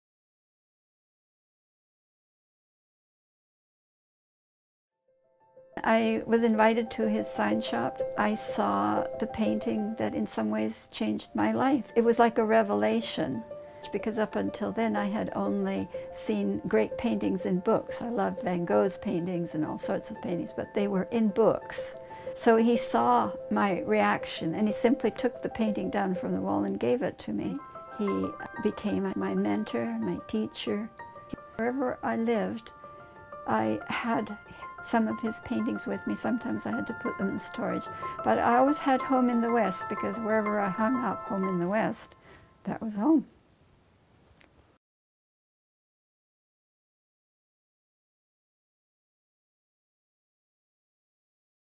Carle Hessay, As I Knew Him - Dr. Maidie Hilmo
Dr. Maidie Hilmo first met Carle Hessay in Langley, BC when she was 16 and waitressed in the Golden Pagoda Cafe after school. Since art was her favourite subject, she started talking to local artist Carle Hessay. He invited her to his sign shop and studio, where she was awed by HOME IN THE WEST. He took it off the wall and gave it to her. This began her life long collection of his work, which continued throughout her subsequent academic career. Stories of Carle’s world travels and his extensive knowledge of the classics shaped her own values and vision of life.

HOME IN THE WEST, painted in 1958 when Carle was finally able to settle down in one place, is imbued with his own yearnings for a home in a small community. BREAK OF DAY was left drying on Carle’s painting table when he died dancing at a New Year’s party on January 31, 1978 at the Sasquatch Inn in Spuzzum, BC.